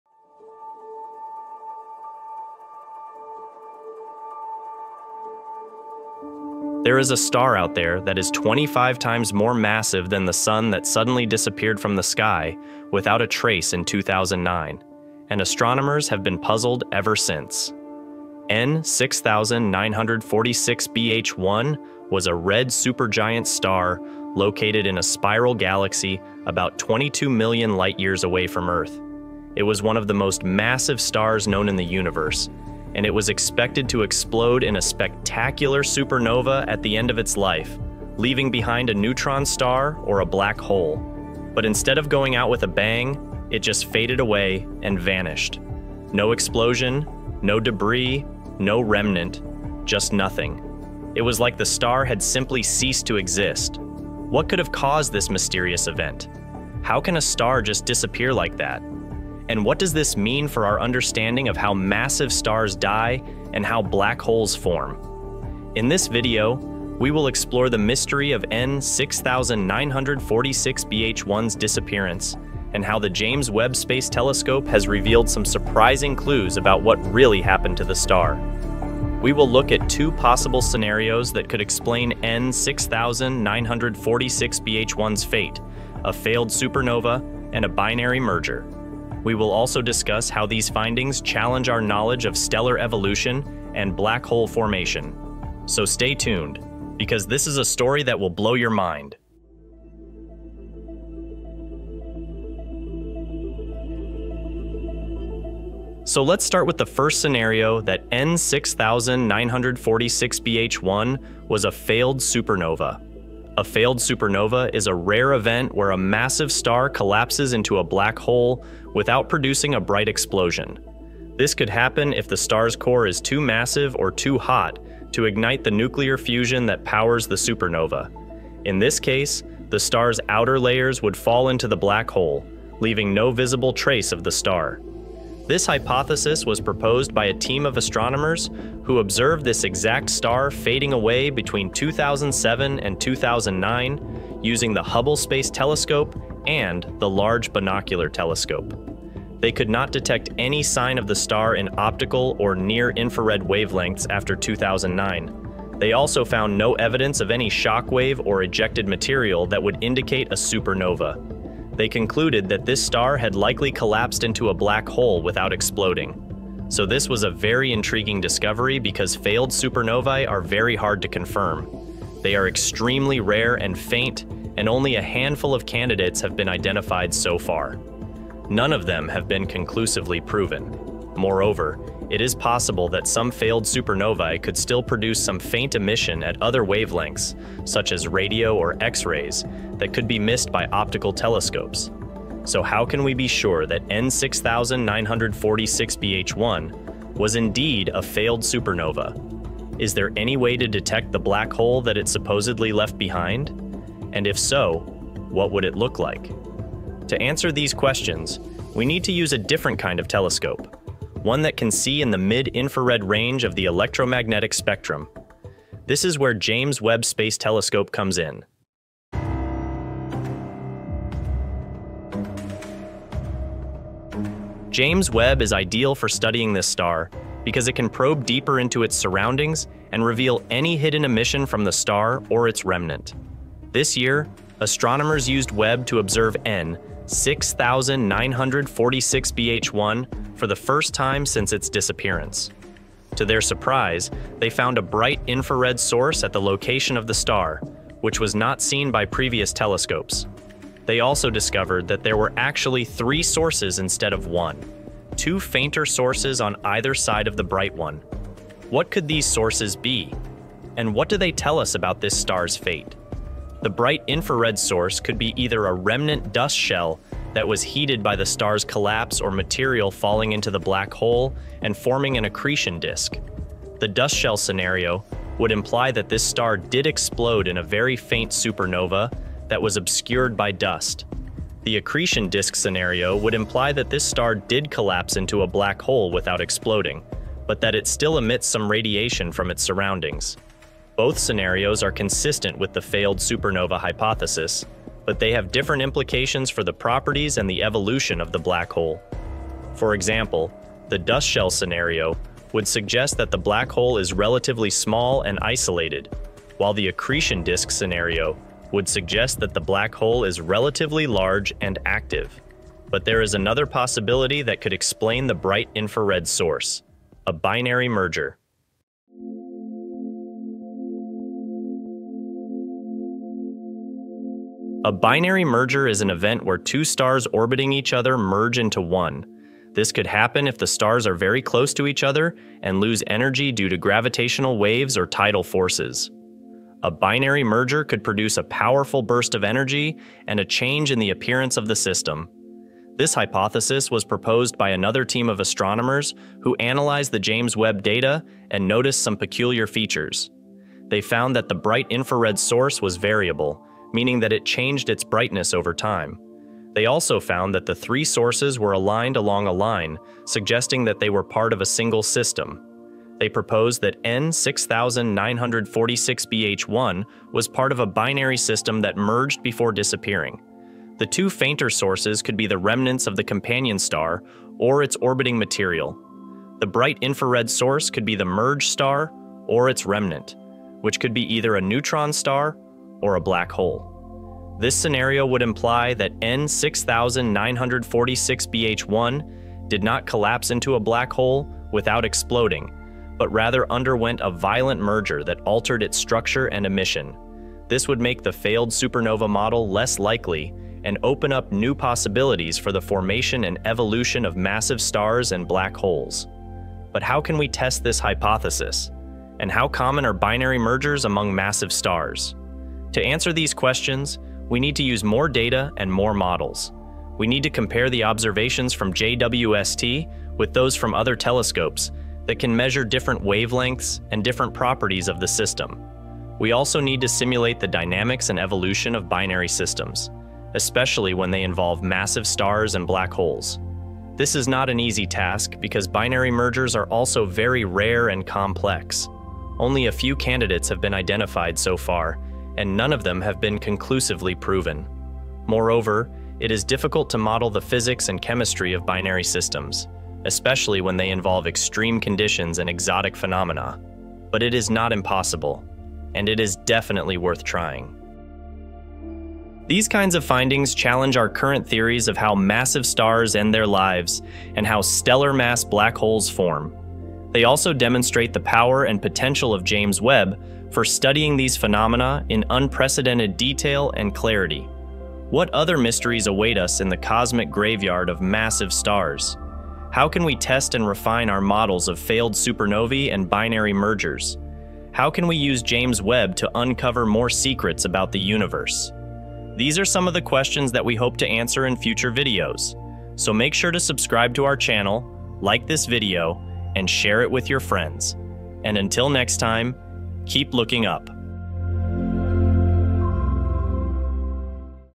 How JWST Solved the Puzzle of a Vanishing Star: N6946-BH1- The Star That Went Missing
In this episode, we explore the mystery of a star that disappeared from the sky in 2009, and how the James Webb Space Telescope (JWST) revealed some surprising clues about its fate. We discuss two possible scenarios: a failed supernova and a binary merger. We also explain the implications and significance of these findings for our understanding of how massive stars die and how black holes form. This is a story that will blow your mind!